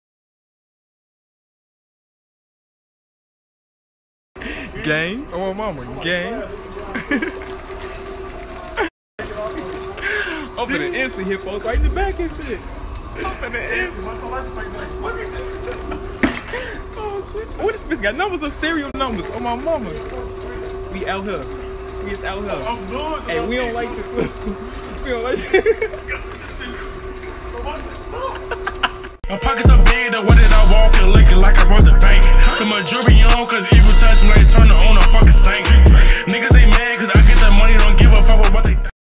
SB DeVinchi & B Don Foolin' Around In Wal Mart Sept. 19, 2018
S.F.S Members SB DeVinchi & B Don Playin Around Driving On Scooters On a Late Night Trip In Wal Mart